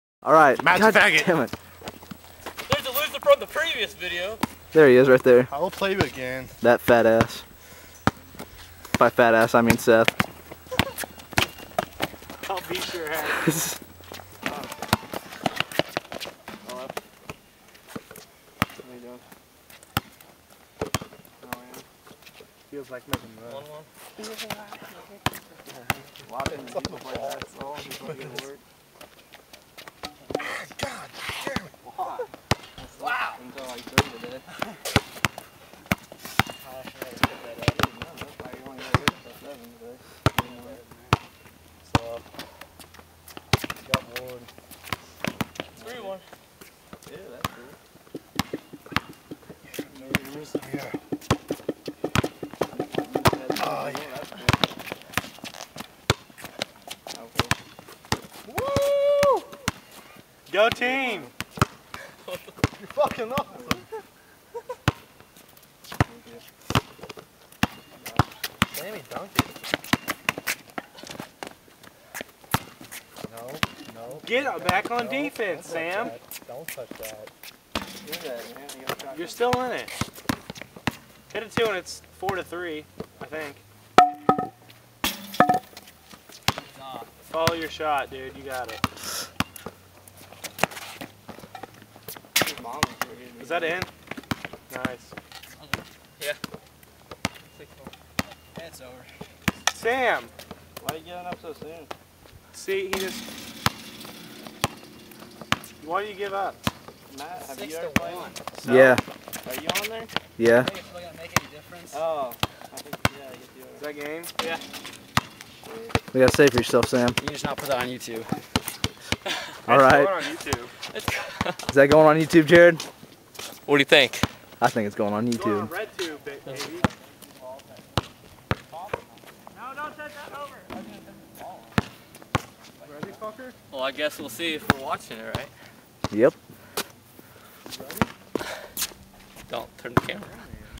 Jerrod Kauffman's 1vs1 YouTube Challenge: Sam Smeltser
Game 5 vs Sam Smeltser

Jerrod Kauffman's 1vs1 YouTube Challenge, Going on all spring/summer long.  See who will be the first to beat me.

Want to challenge me 1vs1? just let me know, anyone can challenge me, ANYONE!!! obviously though this is only for those who actually live in my area (Kahoka, Wayland, Keokuk) I will be doing rematches if and when I start running out of challengers